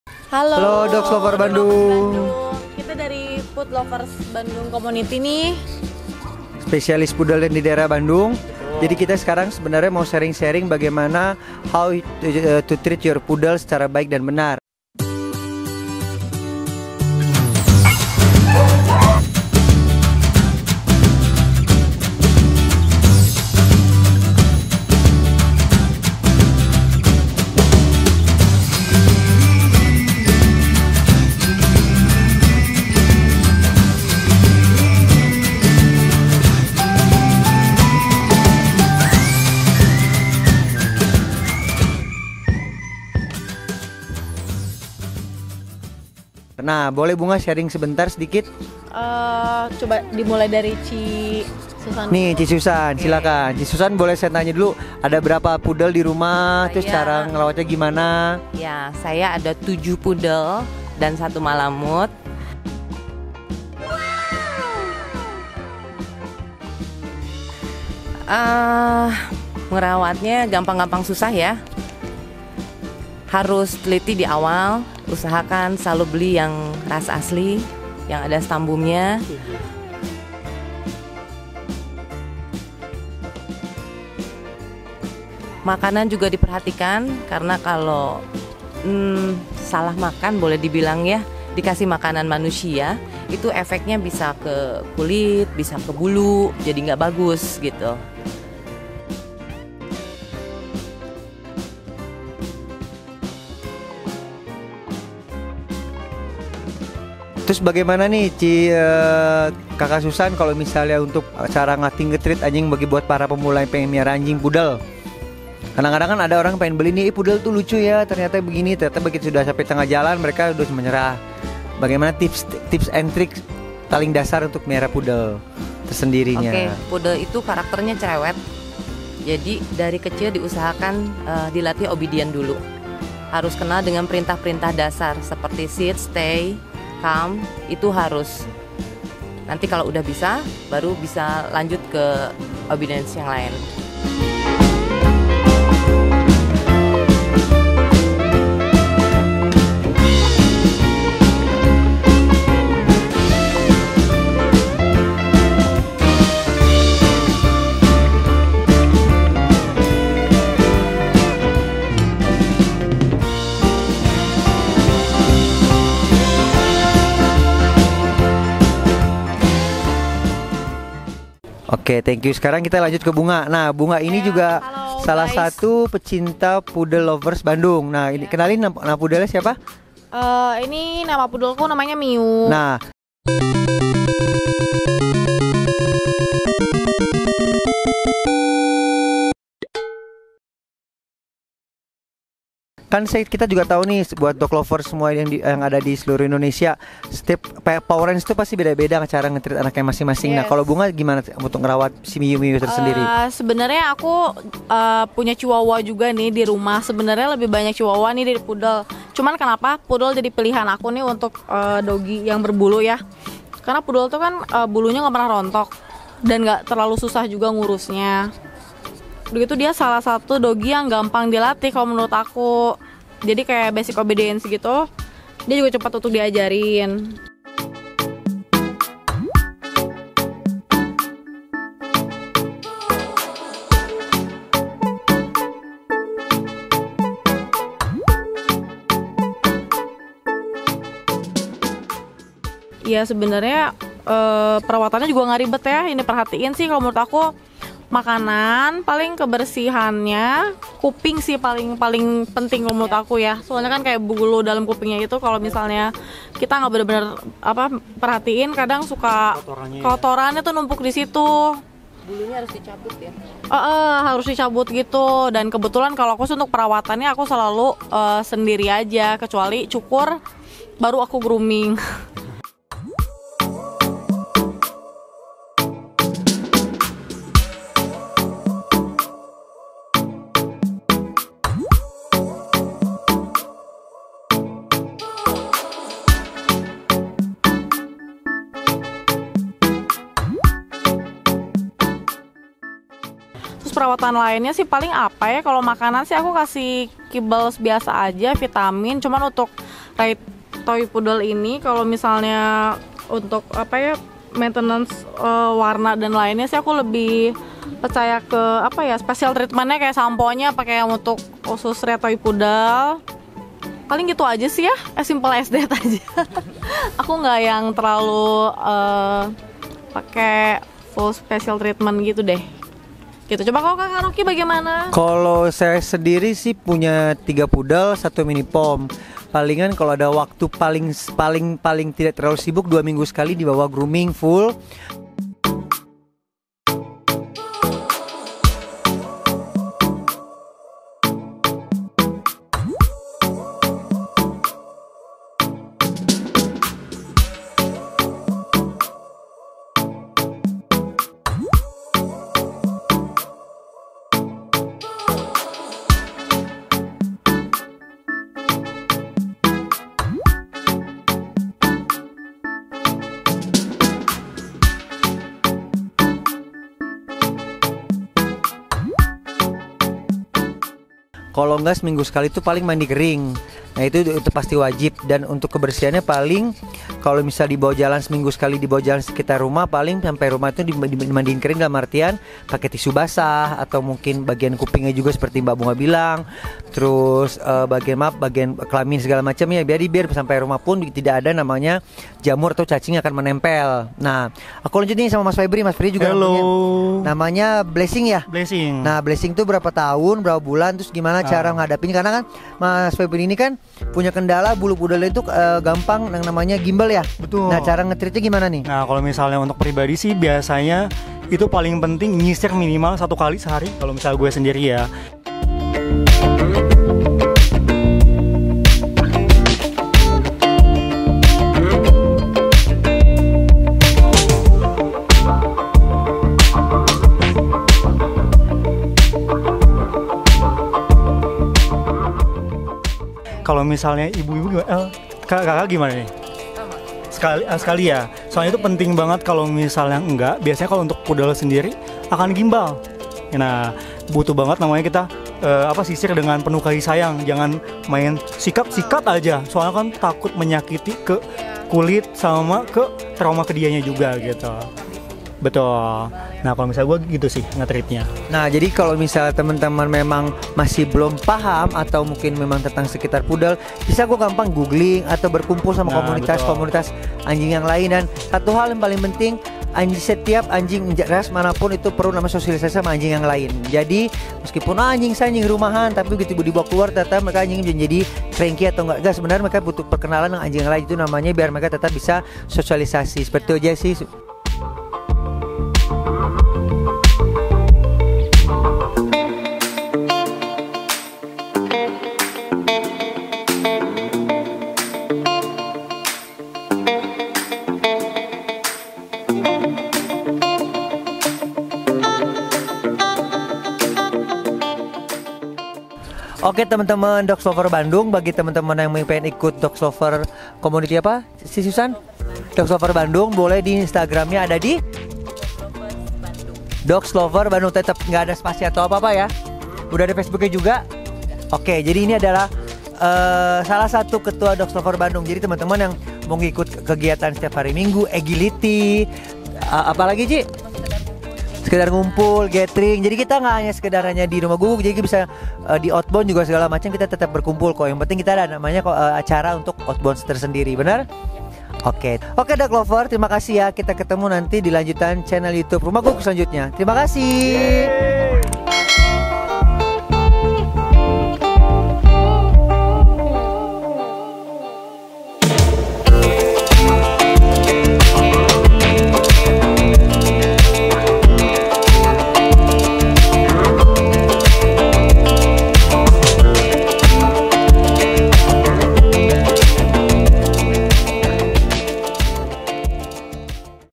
Komunitas Poodle Berbagi Tips Cara Merawat Poodle di Rumah Guguk
Halo DogLovers,
Rumah guguk kedatangan tamu istimewa, komunitas pecinta poodle di Bandung, mereka berbagi tips dan bagaimana cara mereawat poodle yang benar, penasaran kan, yuk tonton videonya sampai habis ya.

Rumah Guguk Bandung
Jl. Pada Lestari No.23 Cidadap, Isola, Kec. Sukasari, Kota Bandung, Jawa Barat 40154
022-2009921